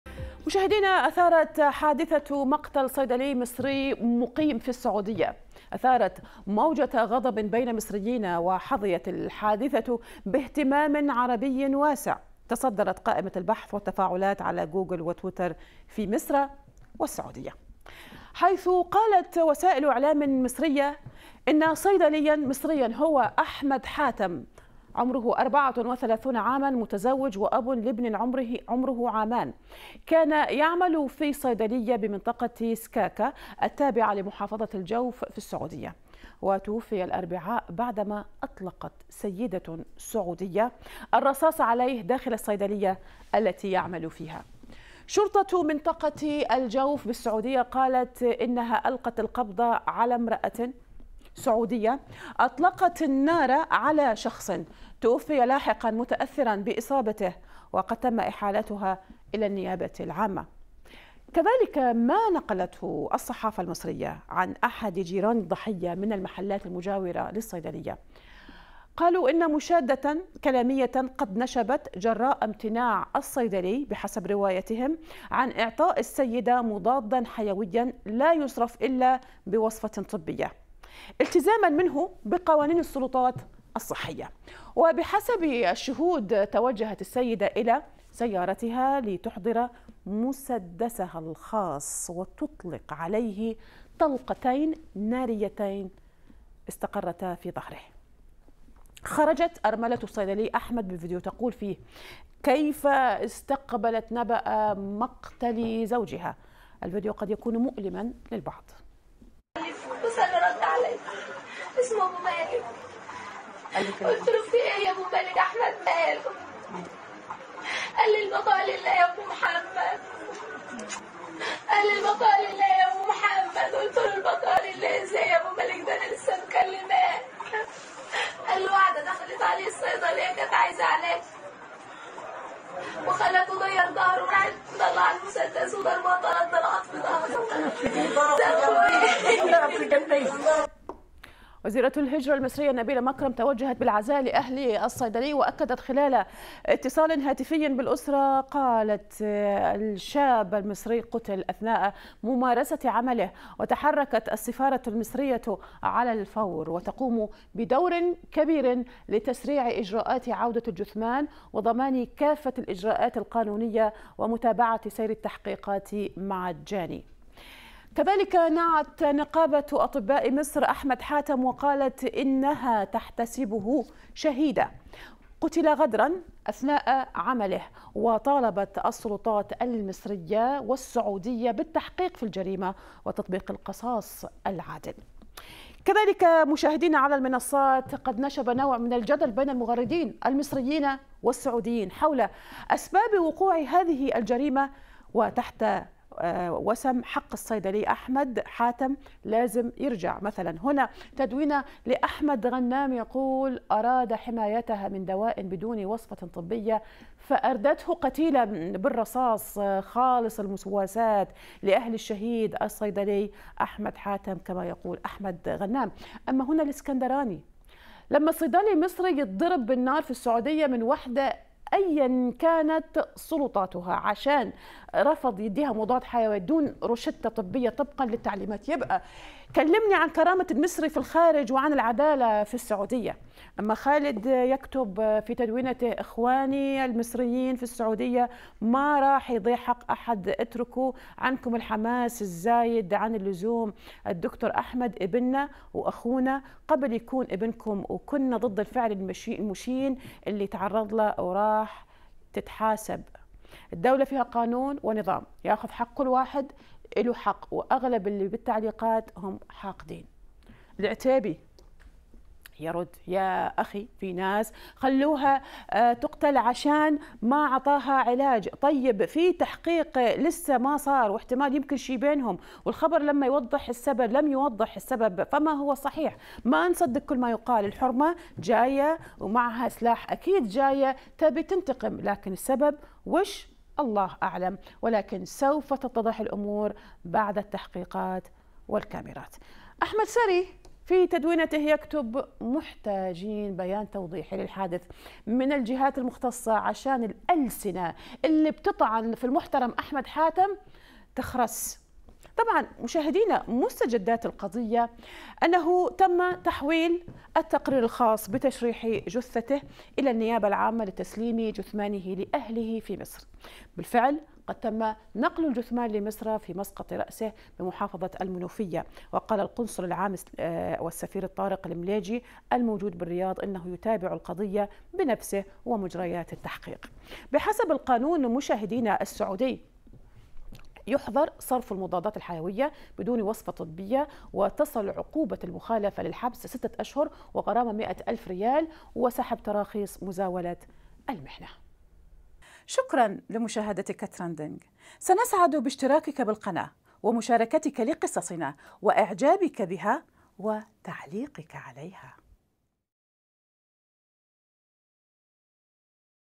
سجال مصري سعودي بعد مقتل صيدلي مصري على يد مواطنة سعودية
مقتل صيدلي مصري على يد مواطنة سعودية
أثارت حادثة قتل الصيدلي المصري أحمد حاتم (34 عاما) كان يعمل في صيدلية بمنطقة سكاكا، التابعة لمحافظة الجوف في المملكة العربية السعودية، وتوفي عقب إطلاق سيدة سعودية الرصاص عليه داخل الصيدلية التي يعمل بها، ما تسبب في سجالا إلكترونيا بين مصريين وسعوديين عبر مواقع التواصل الاجتماعي، وسط دعوات لتهدئة المشاعر ونبذ أية مسببات للشقاق بين الشعبين.